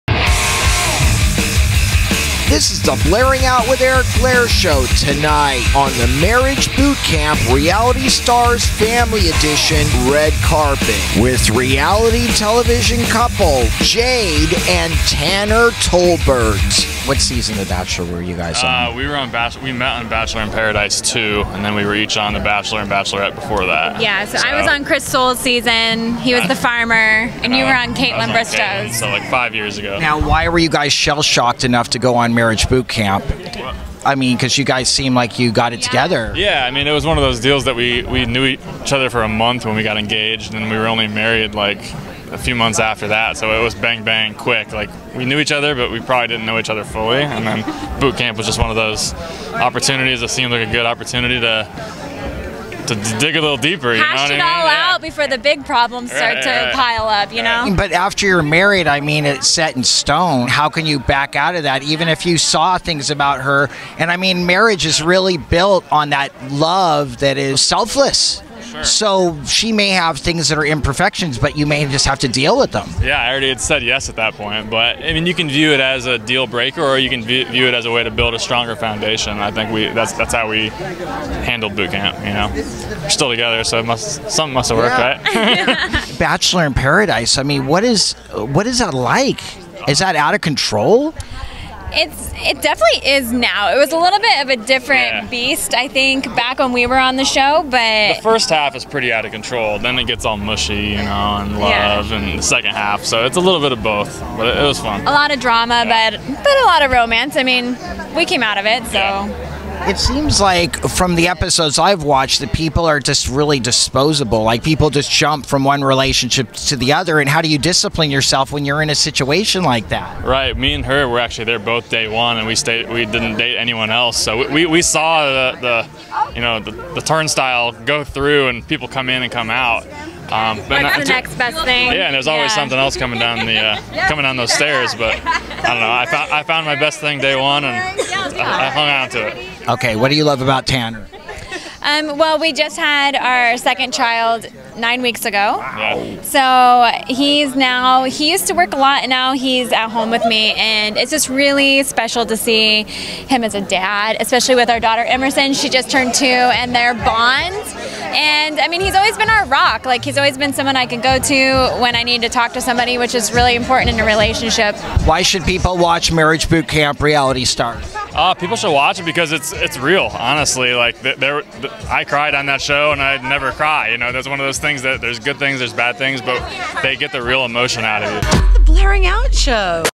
The Bachelor's TANNER AND JADE talk MARRIAGE BOOTCAMP 2019
The Bachelor's TANNER AND JADE talk  MARRIGE BOOTCAMP 2019